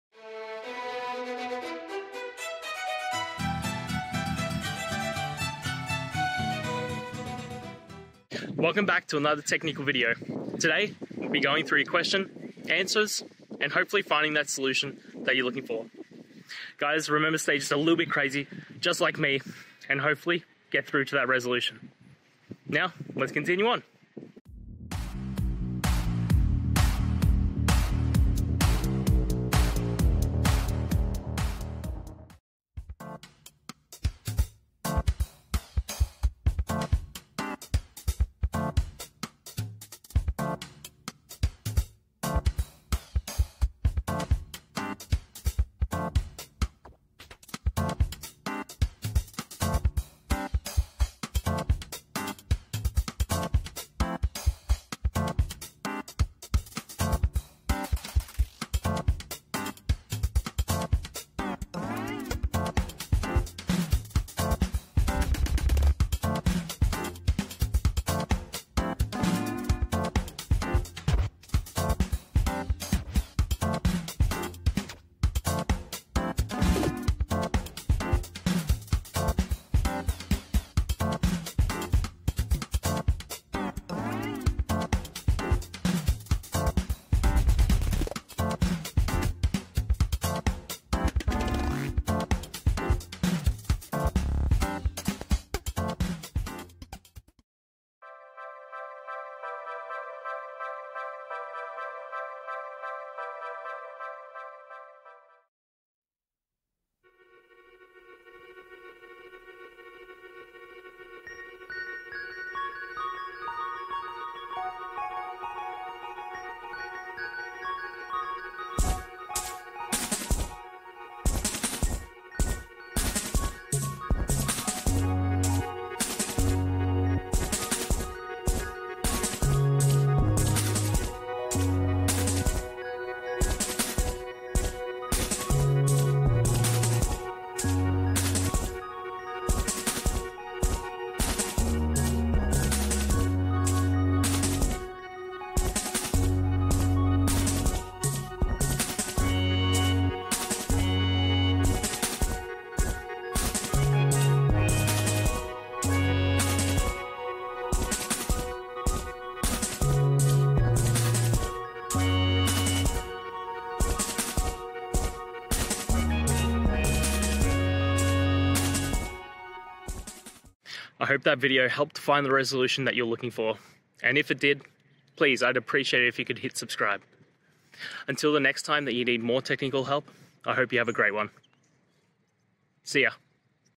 Join data.table in R by Sampling: A Guide to Efficient Data Merging
In this video, we delve into the powerful capabilities of the data.table package in R, focusing on the art of efficient data merging through sampling techniques. Whether you're working with large datasets or seeking to optimize your data manipulation processes, this guide will provide you with practical insights and strategies to enhance your workflow. Join us as we explore how to leverage sampling to streamline your data.table operations and improve performance in your R projects.

Today's Topic: Join data.table in R by Sampling: A Guide to Efficient Data Merging

Related to